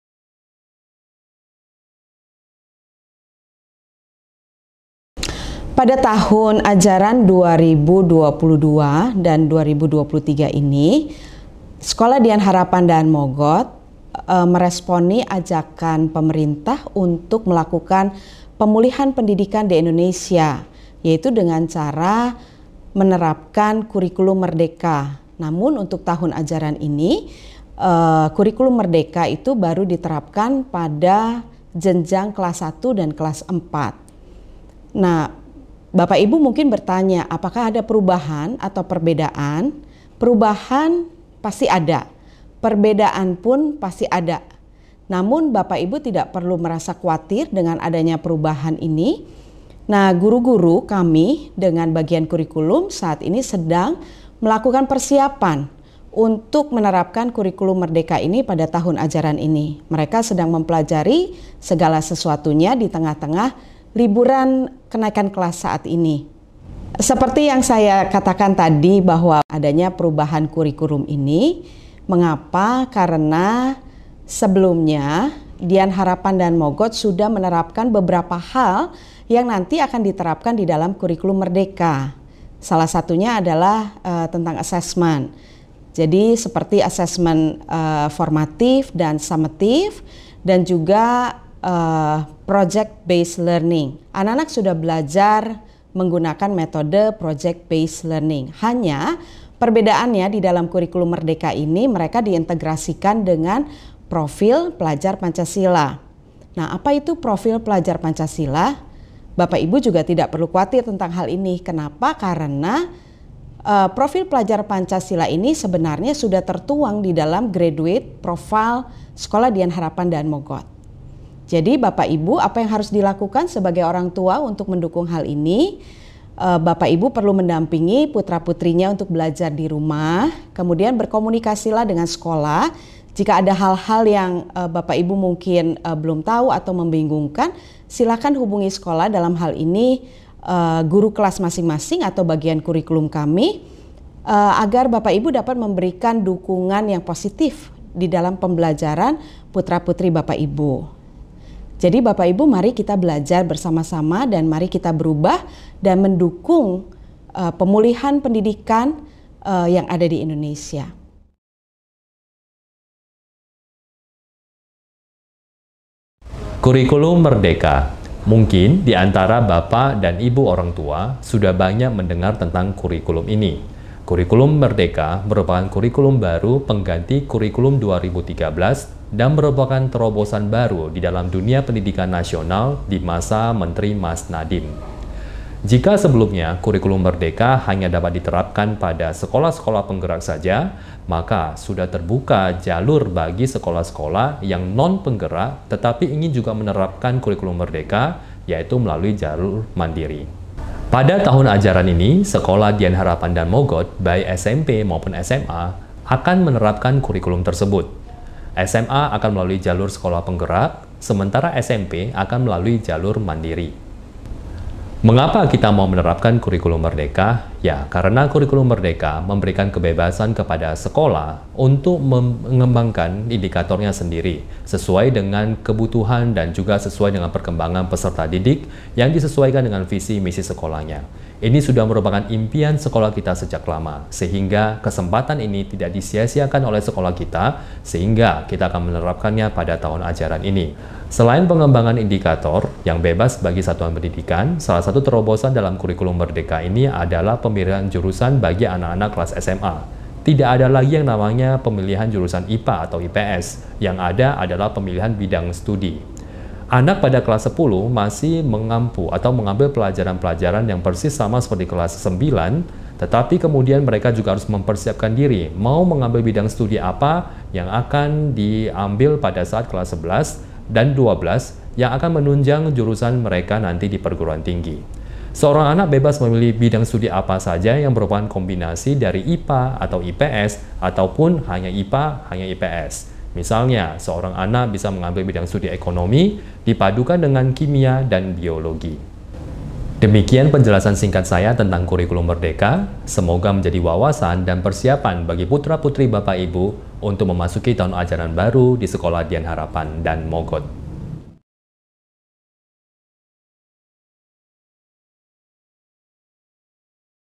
Kurikulum Merdeka Sekolah Dian Harapan Daan Mogot
Penjelasan tentang Kurikulum Merdeka oleh Junior dan Senior Principal. Yuk, kita simak !